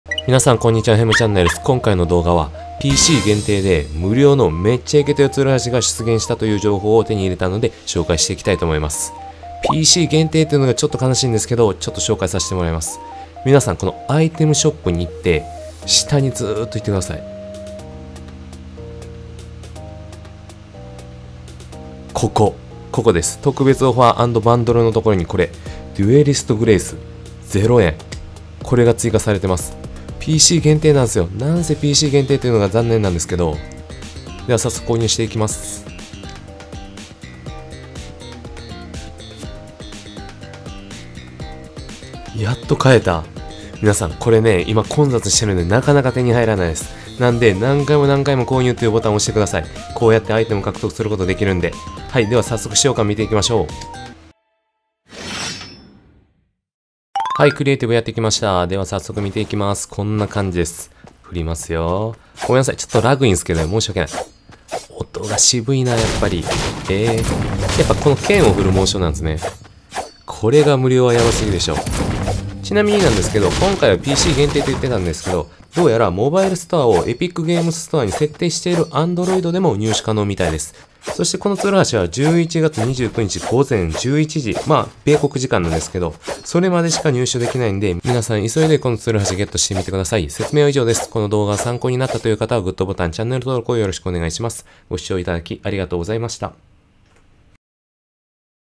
これがタダ！？無料ツルハシ｢デュエリストグレイス｣の入手方法の紹介！※バグって買えない時の対処法、使用感の紹介もあります。
➳クリエイターサポート
HEMCH